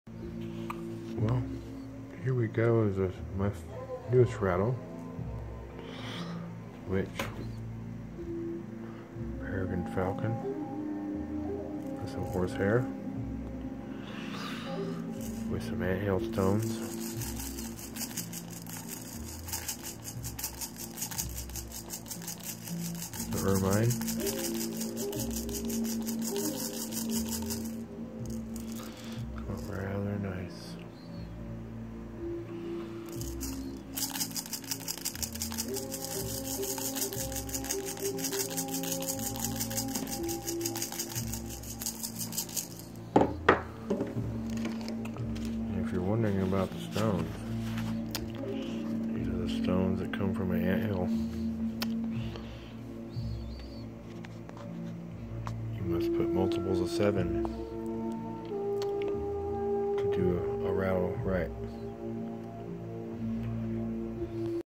Painted turtle shell rattle
This is a rattle with our minds fur deerskin and peregrine falcon feathers with anthill rocks